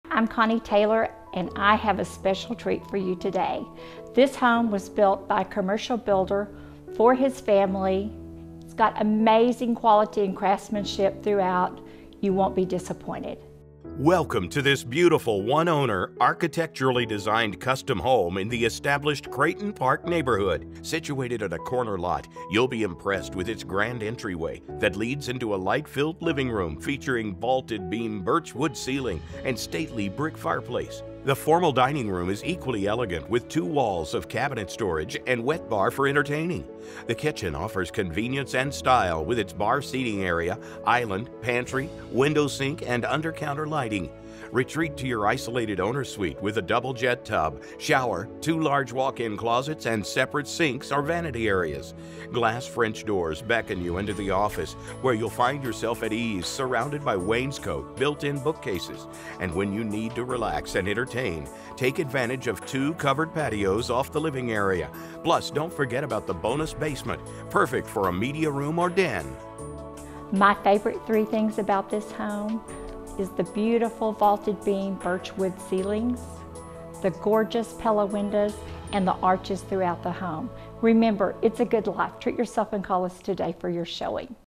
For Sale, Creighton Park, 7304 Stacy, Amarillo, TX by Connie Taylor
Welcome To This Beautiful One Owner, Architecturally Designed Custom Home in The Established Creighton Park Neighborhood! Situated on a corner lot, you'll be impressed with its grand entryway that leads into a light-filled living room featuring vaulted beam birch wood ceiling & stately brick fireplace. The formal dining room is equally elegant with two walls of cabinet storage & wet bar for entertaining. The kitchen offers convenience & style with its bar seating area, island, pantry, window sink & under counter lighting. Retreat to your isolated owner suite with a double jet tub, shower, two large walk-in closets & separate sinks/vanity areas. Glass French doors beckon you into the office, where you'll find yourself at ease surrounded by wainscot, built in bookcases. And when you need to relax and entertain, take advantage of two covered patios off the living area. Plus, don't forget about the bonus basement - perfect for a media room or den!